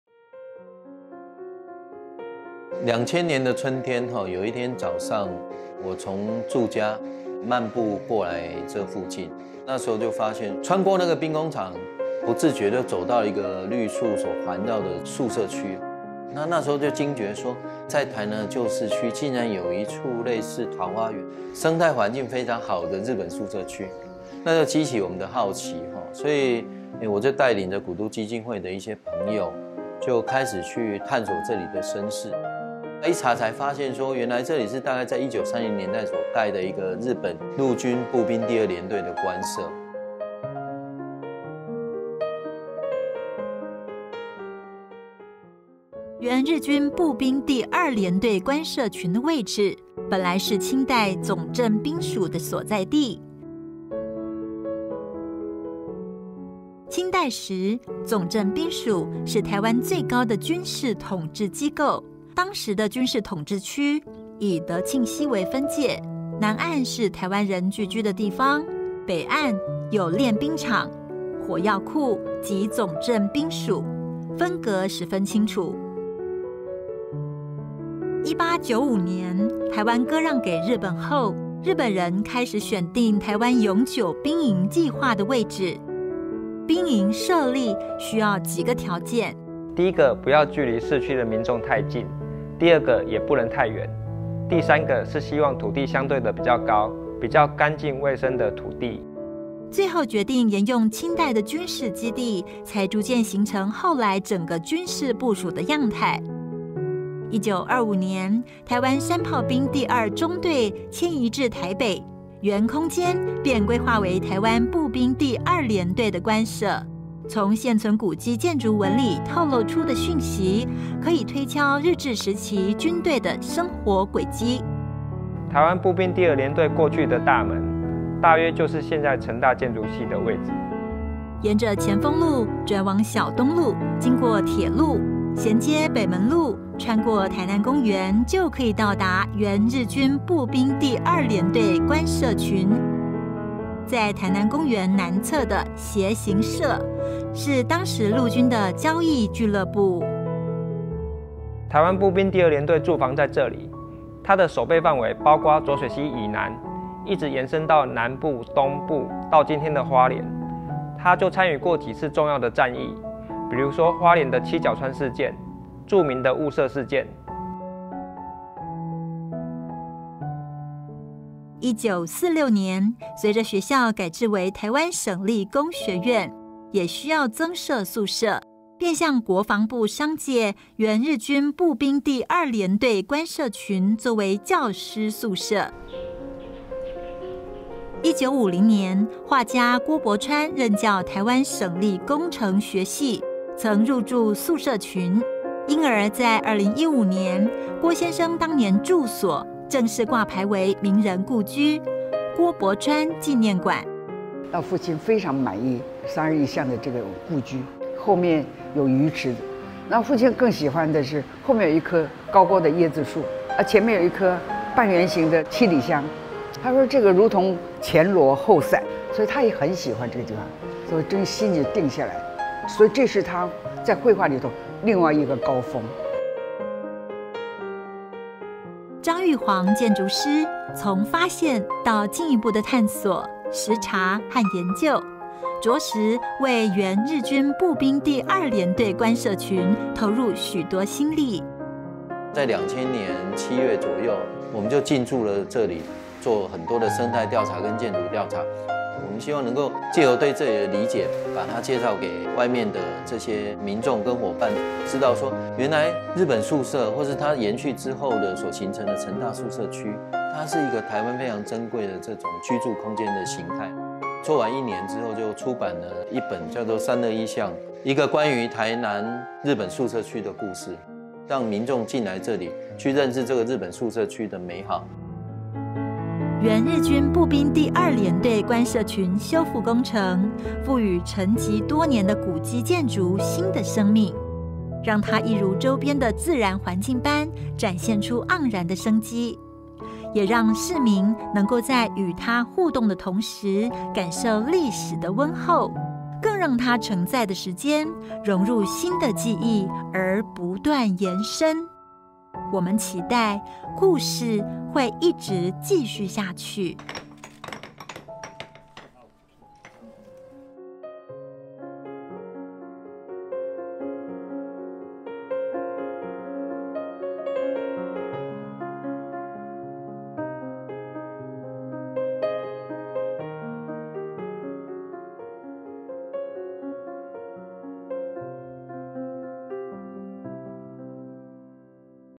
穿越時光的桃花源：321巷日式宿舍群修復工程
位於臺南市北區公園路321巷內的日式宿舍群是臺南的市定古蹟，正式古蹟名稱為「原日軍步兵第二聯隊官舍群」。影片以「穿越時光的桃花源」為題，看321巷內的日式宿舍群從日治時期的軍官住所、戰後成功大學教職員宿舍、藝術聚落到2020年展開古蹟修復工程前的發展與演變。